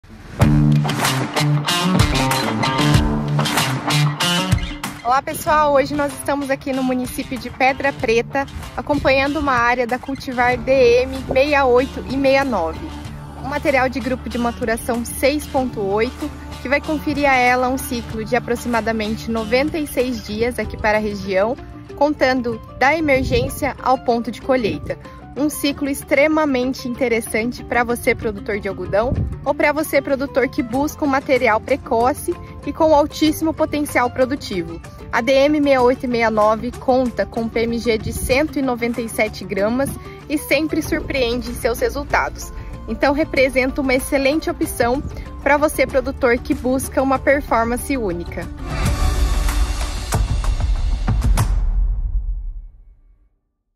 DM68I69 IPRO | Pedra Preta - MT
Diretamente de Pedra Preta - MT, a nossa supervisora de desenvolvimento de mercado, Bruna da Silva, apresenta a cultivar DM68I69 RSF IPRO, a escolha ideal para quem busca alto desempenho e rentabilidade na produção.

Eleve a produtividade do seu plantio com a DONMARIO!

Quer saber mais sobre essa cultivar?